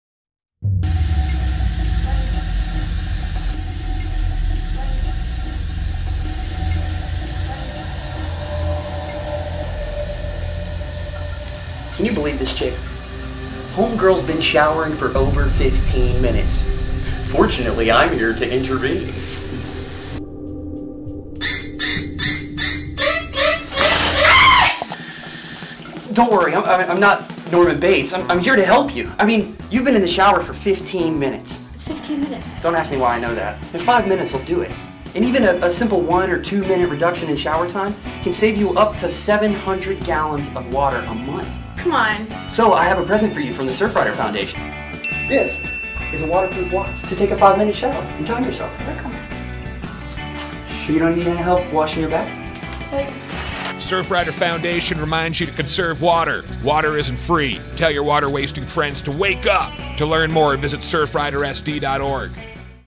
know your H2O - shower scene.
SURFRIDER water conservation campaign.

episode three:  the shower.

starring marty benson and chris cantore.

a cantore creative/NAS production.